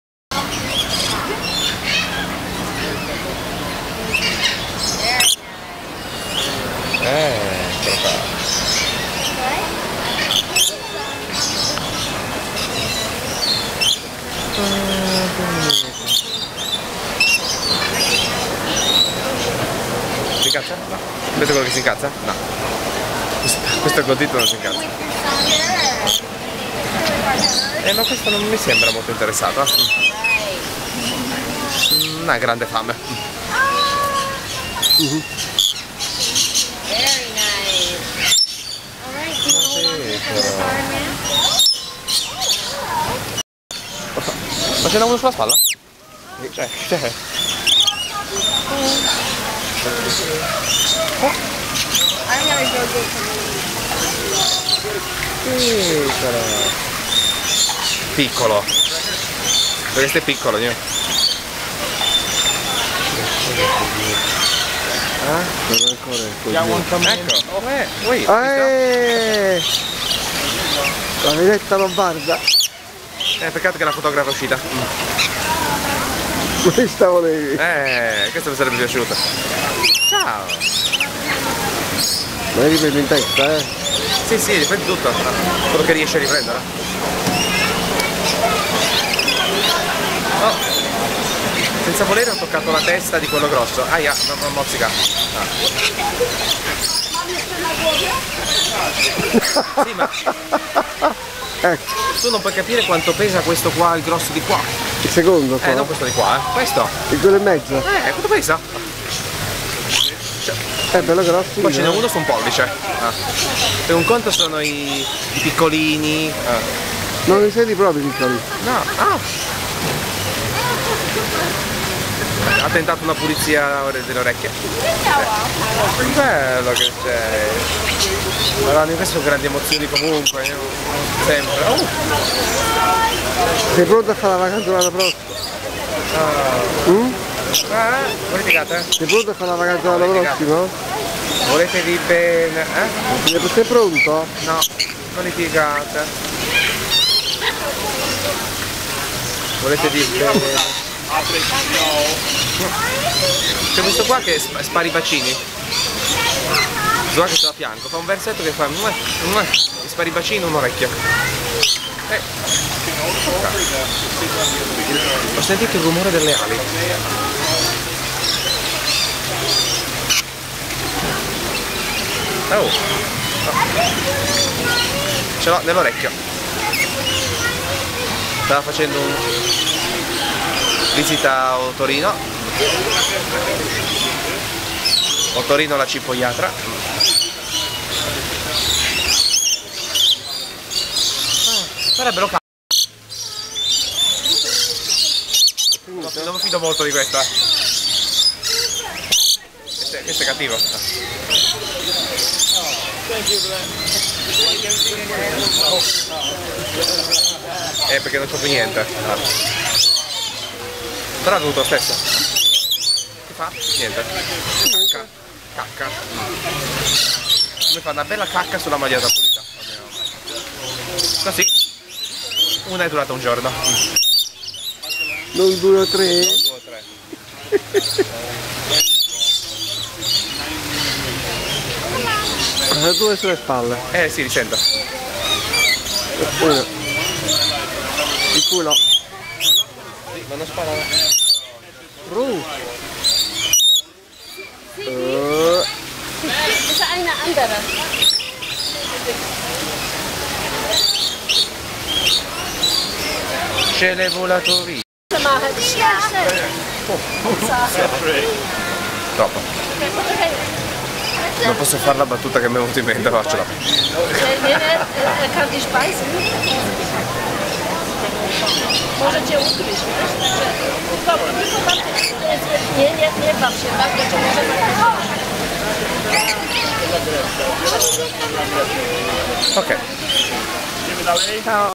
Pappagalli affamati... Marzo 2015 - Lory Landing - Busch Gardens Tampa
Come sempre, eccomi in compagnia dei miei amati pappagalli del Lory Landing al Busch Gardens di Tampa. Passano gli anni, ma l'amore per loro non cessa di esistere